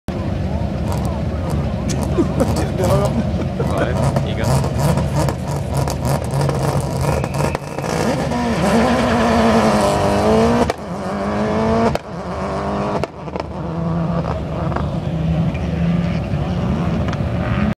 Kevin.p croft christmas stages 2015
Kevin Proctor at the start of leg 2 on the reverse circuit at the croft christmas stages 2015